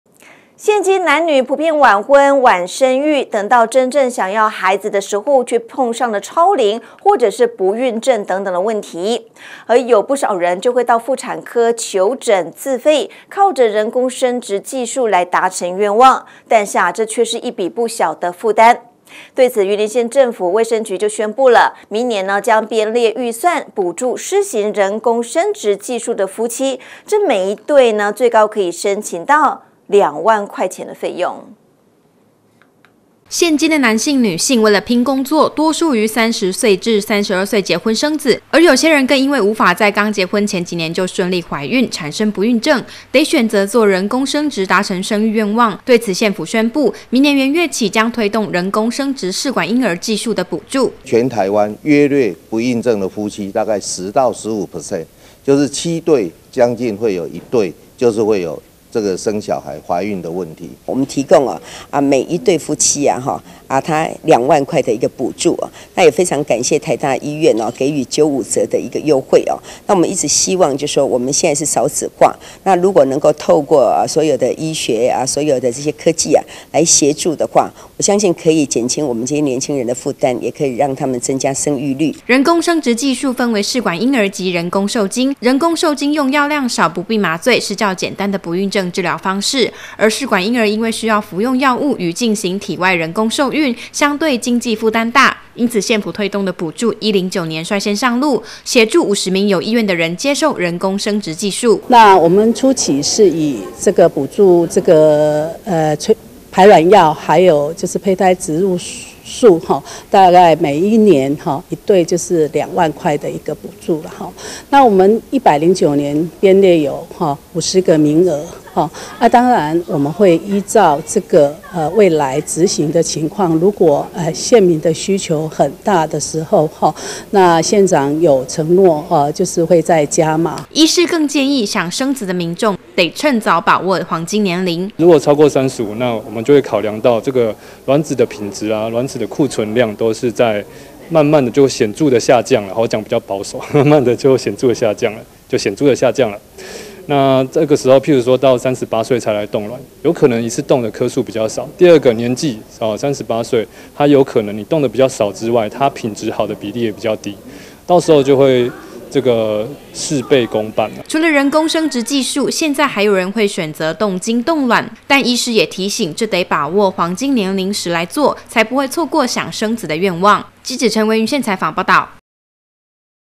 雲林人工生殖補助 減輕不孕夫妻負擔
現在社會上的男女普遍有晚婚、晚生育現象，等到真正想要孩子時，卻碰上超過適合年齡、產生不孕症等問題。因此有不少人會到婦產科求診自費，靠人工生殖技術達成願望，但經費可是一筆不小負擔。對此，雲林縣衛生局宣布，明年將編列預算，補助施行人工生殖技術的夫妻，每一對可以申請最高兩萬塊的補助金。